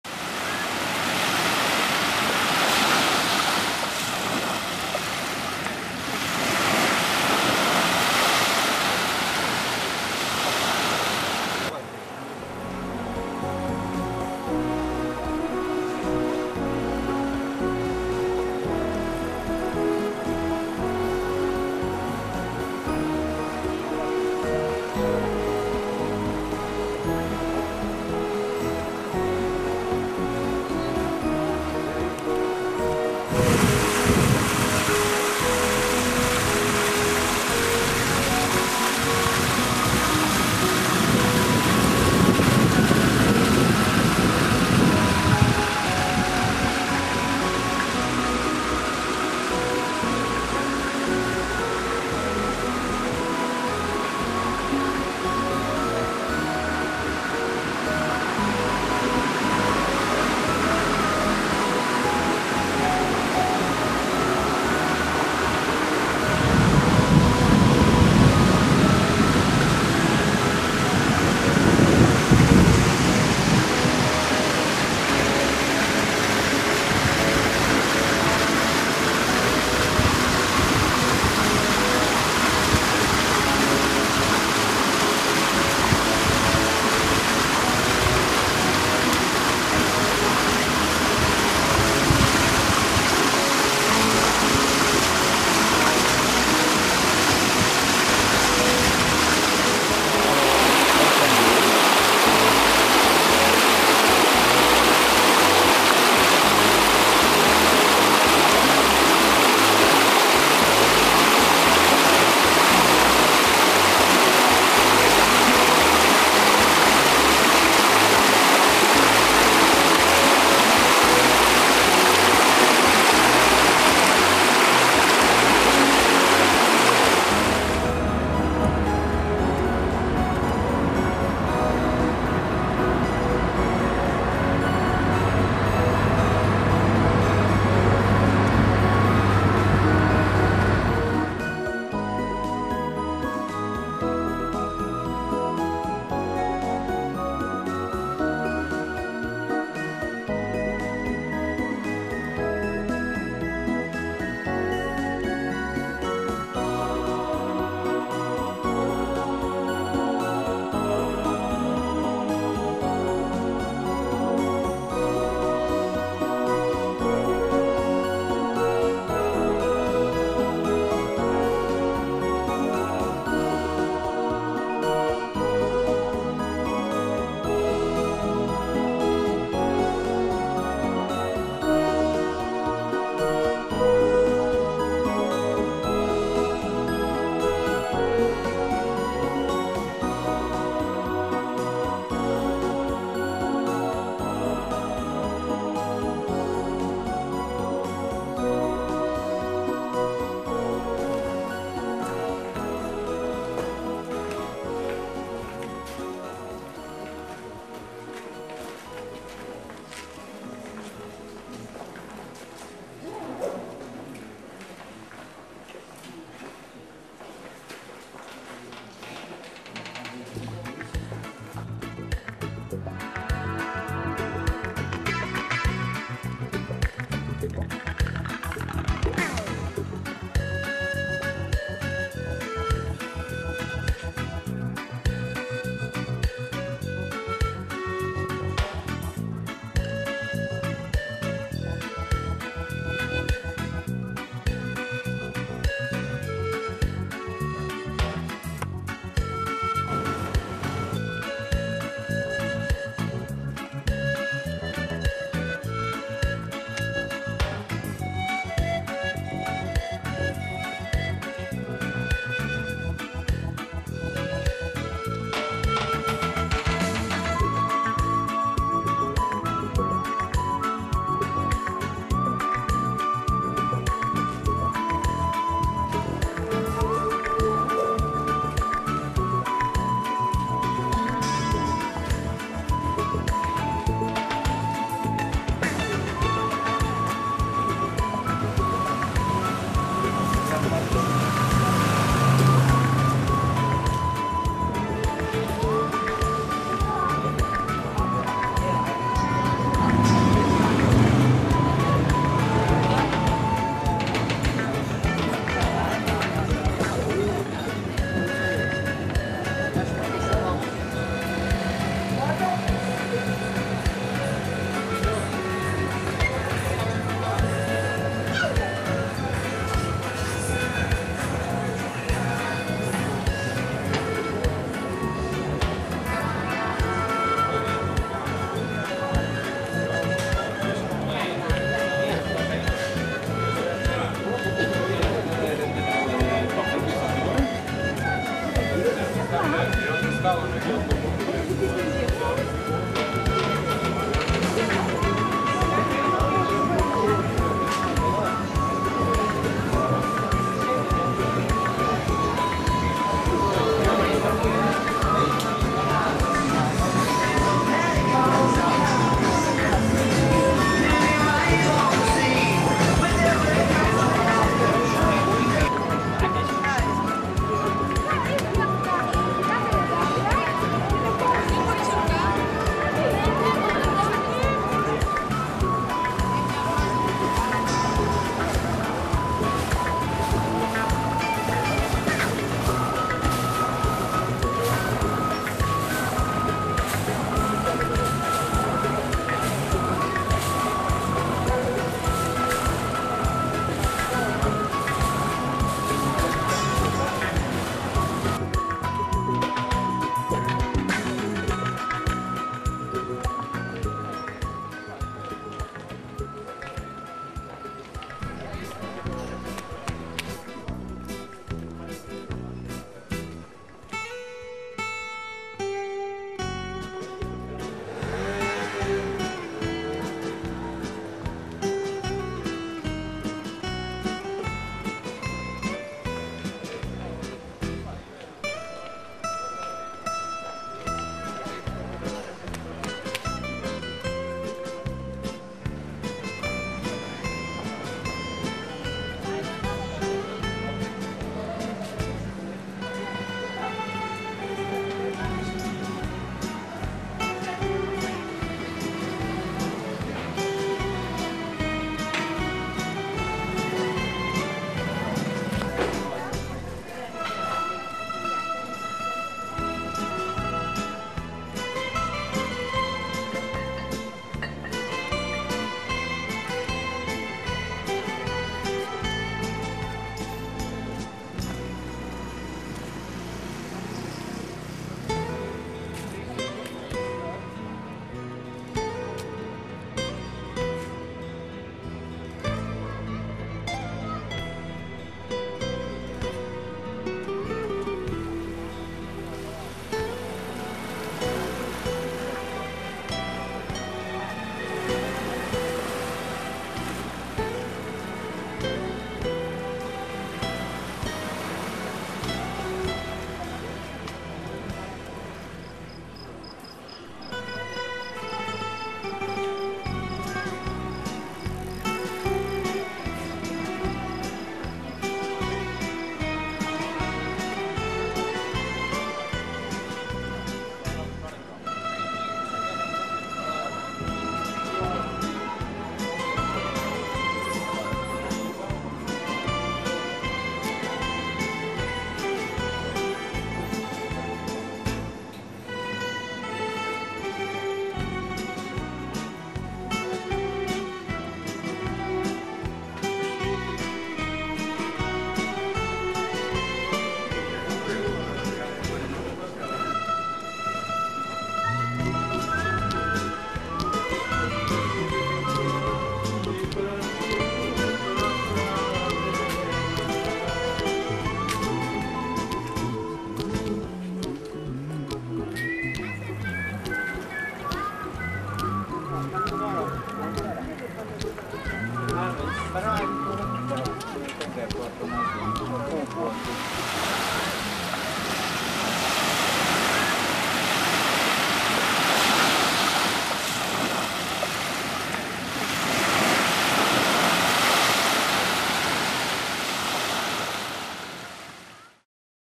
Parga en omgeving Epirus
Tweede deel van onze vakanti- video  over Parga en omgeving Epirus.